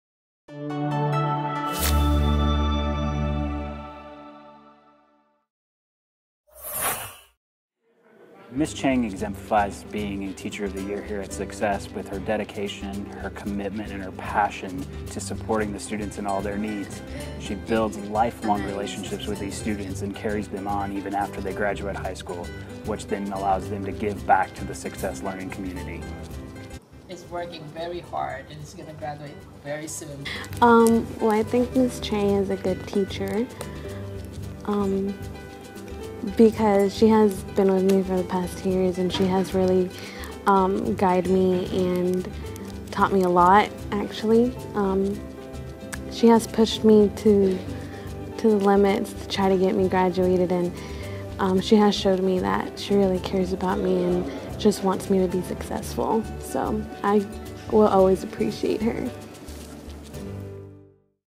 Success High School 2017 Teacher of the Year: Catherine Chang
Round Rock ISD congratulatesCatherine Chang for being named Success High School 2017 Teacher of the Year.